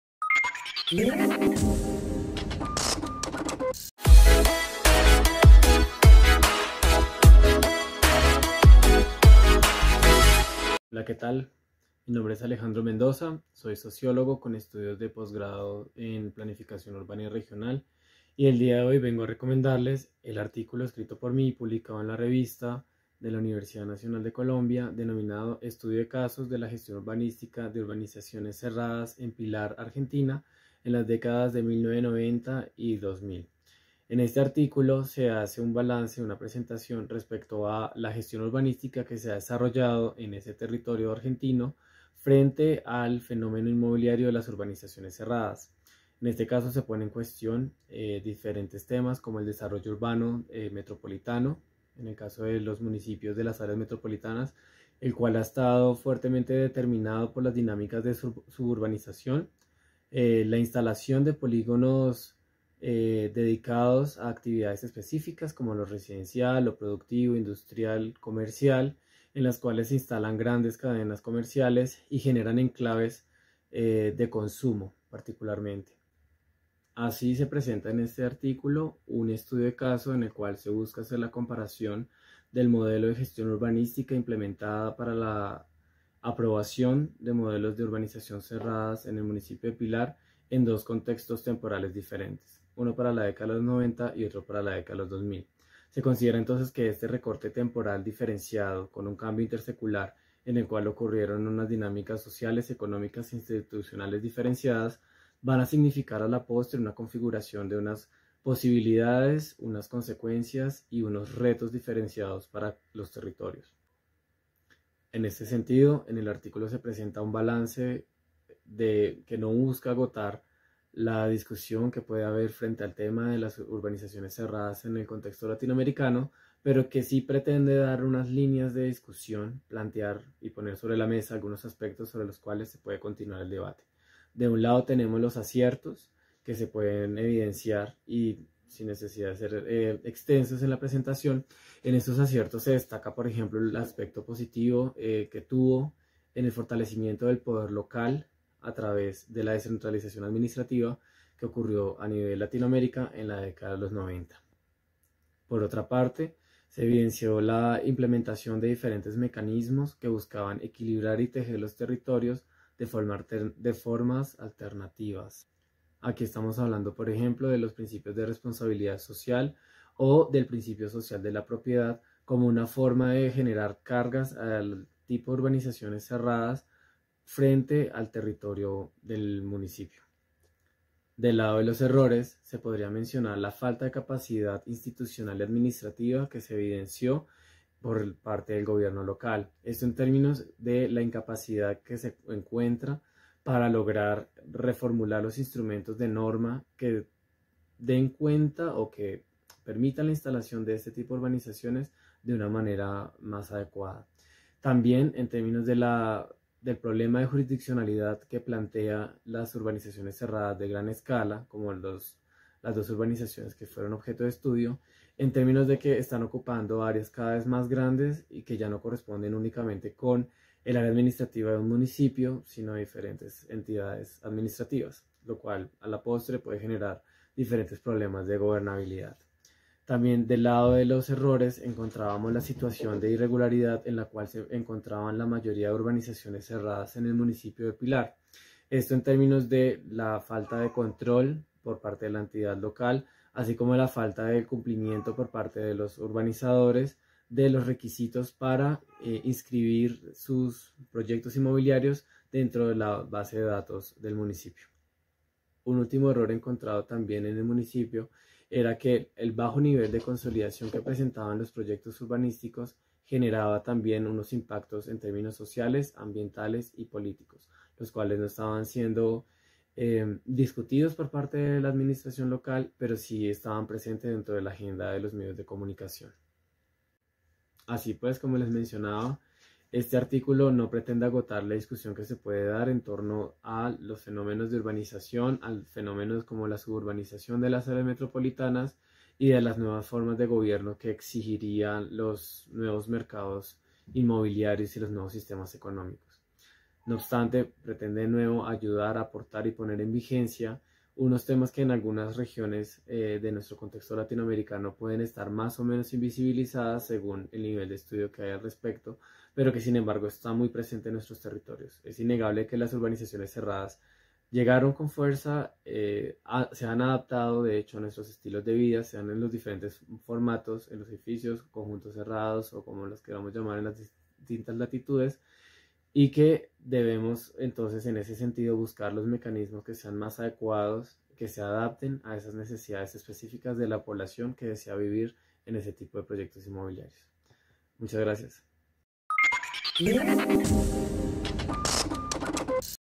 YoRecomiendo un artículo sobre gestión urbanística
El sociólogo Alejandro Mendoza Jaramillo, recomienda su artículo “Estudio de casos de la gestión urbanística de urbanizaciones cerradas en Pilar-Argentina: años 1990 – 2010”, publicado en la Revista "Ciudades, Estados y Política", del IEU de la UNAL.